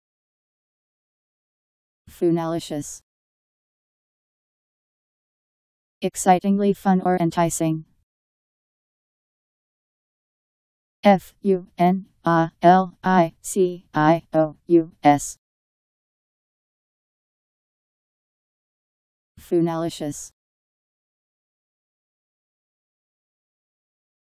Funalicious Meaning
Video shows what funalicious means. Excitingly fun or enticing..  Funalicious Meaning. How to pronounce, definition audio dictionary. How to say funalicious. Powered by MaryTTS, Wiktionary